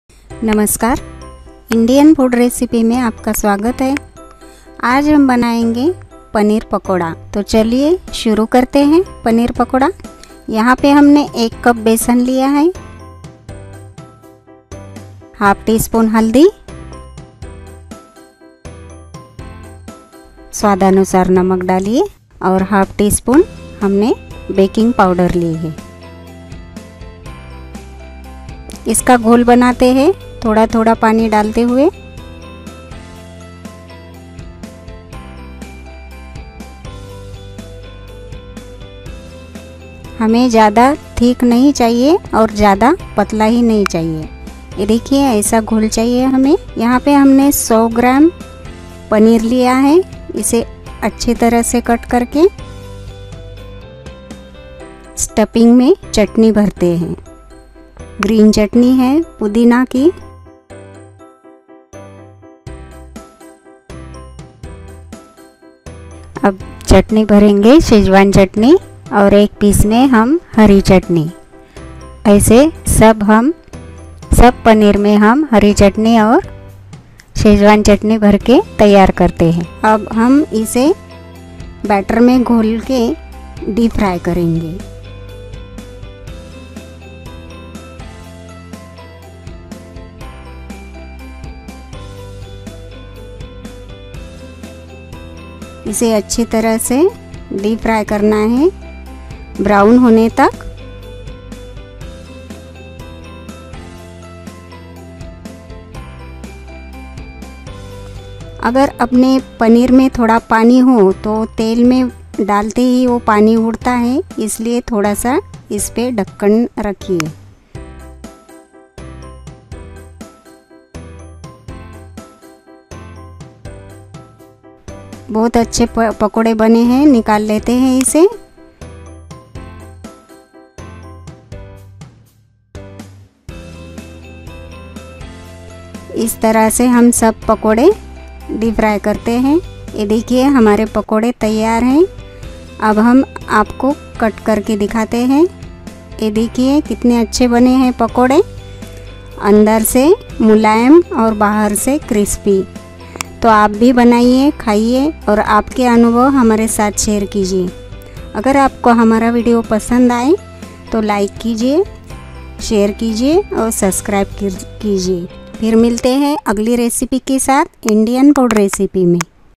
Paneer pakode Recipe | Paneer ke pakode | Crispy paneer pakode
How to make paneer pakode is very tasty snack recipe and very healthy and tasty recipe in 5mint